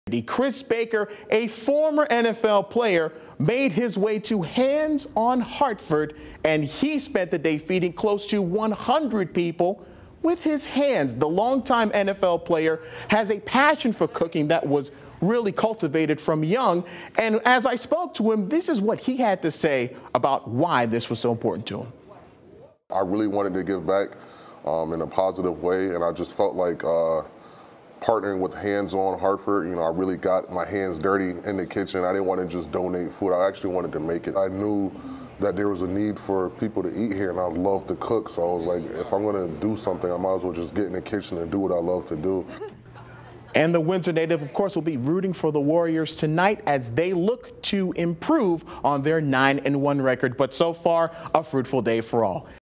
Former NFL player returns to Windsor to volunteer at Thanksgiving food drive
Former NFL player and Windsor native Chris Baker returned to his hometown to serve some meals ahead of Thanksgiving.